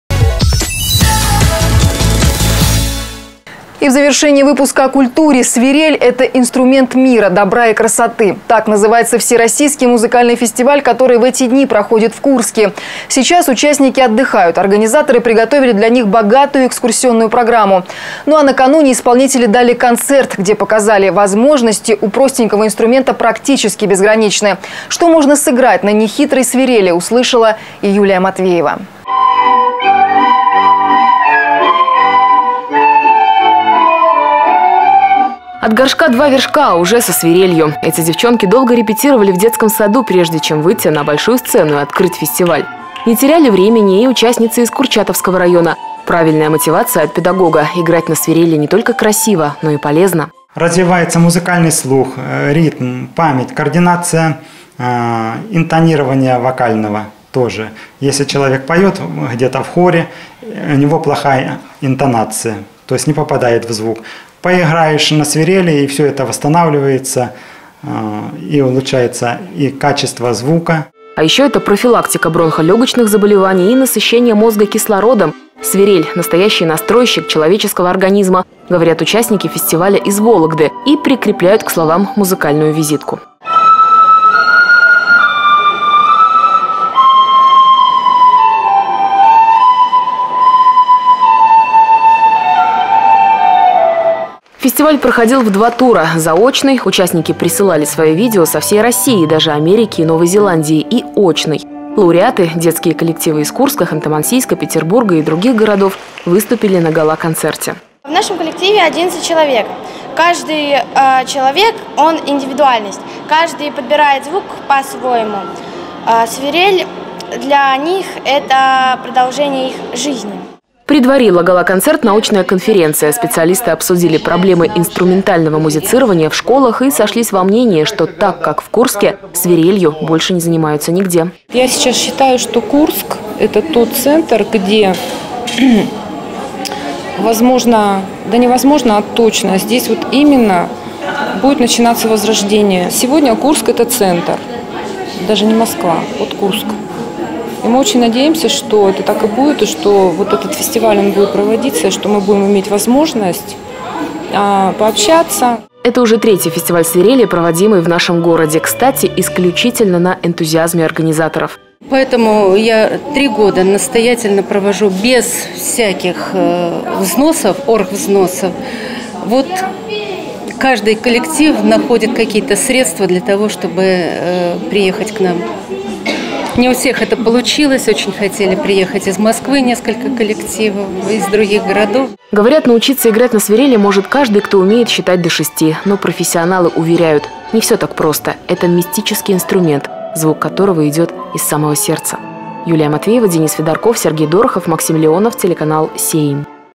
В Курске звучат свирели
«Свирель — это инструмент мира, добра и красоты». Так называется всероссийский музыкальный фестиваль, который в эти дни проходит в Курске. Сейчас участники отдыхают, организаторы приготовили для них богатую экскурсионную программу. А накануне исполнители дали концерт, где показали: возможности у простенького инструмента практически безграничны.